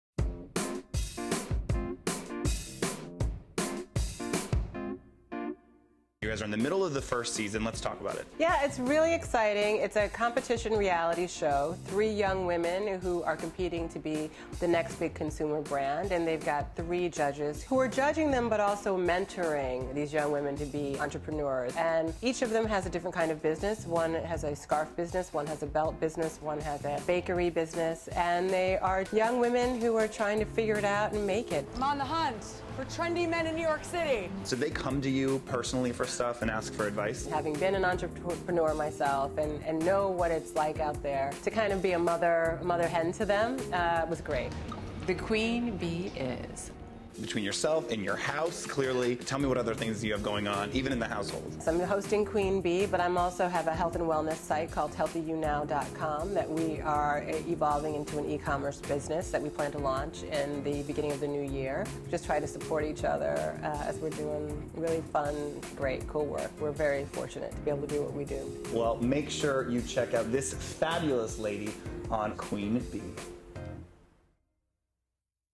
Tonya Lewis Lee Chats with OK! TV
OK! TV's Josh McBride sat down with Tonya Lewis Lee to chat about her new TV show, Queen Bee.